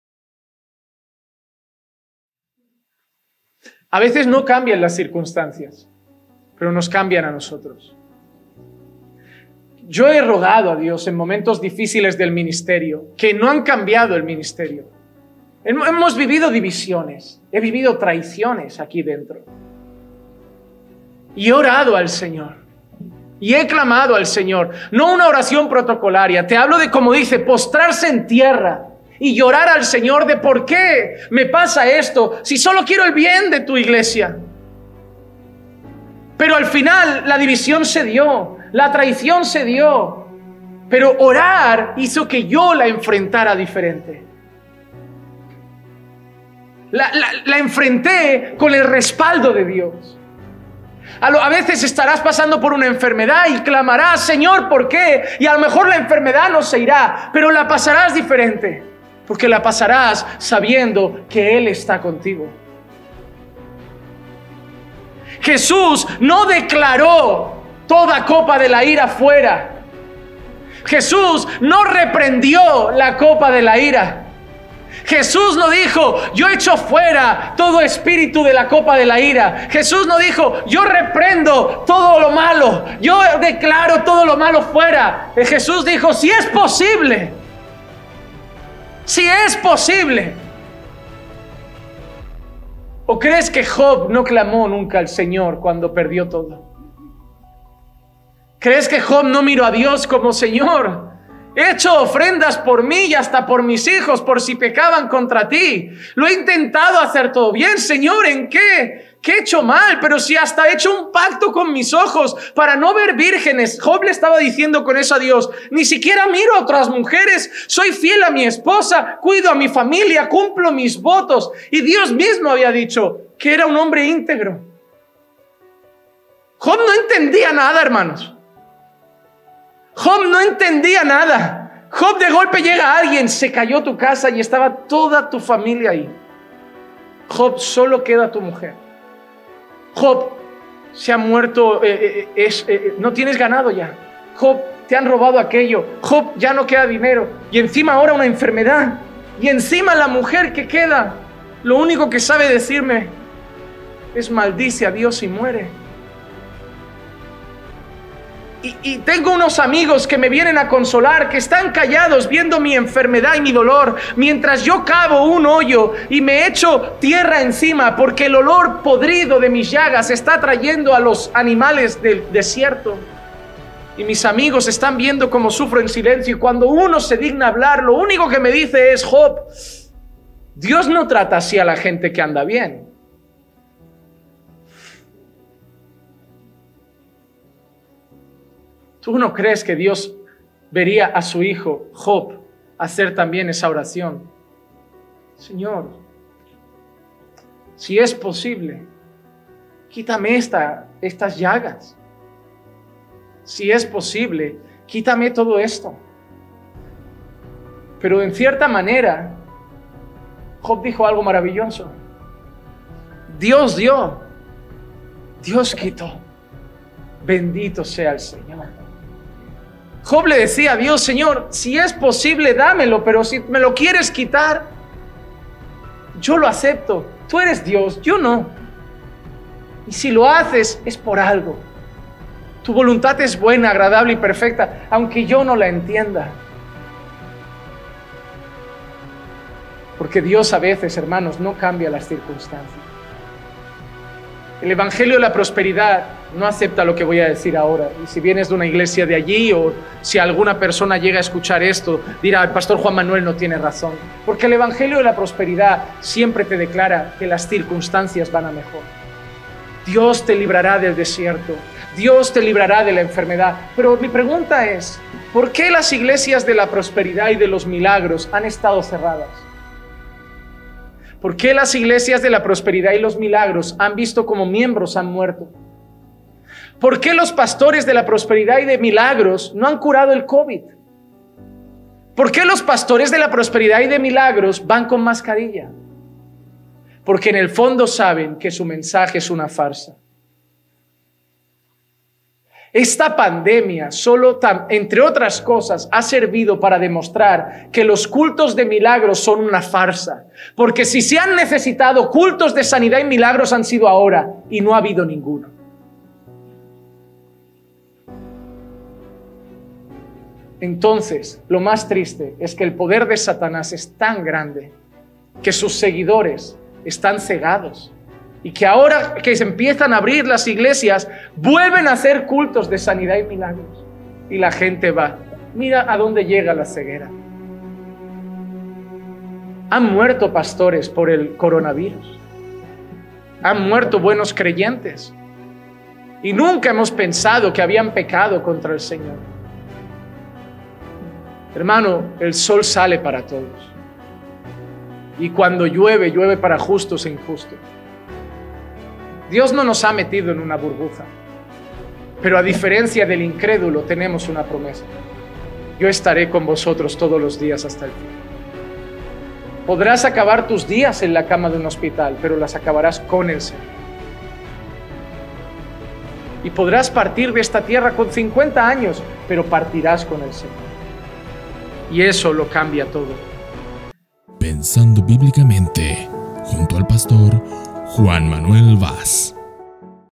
¿Y los Milagreros y Sanadores Durante la Pandemia? - Juan Manuel Vaz
¿Dónde estaban los milagreros y los cultos de milagros ahora que tanta falta hacían? A pesar de todo esto...la ceguera de muchos seguirá...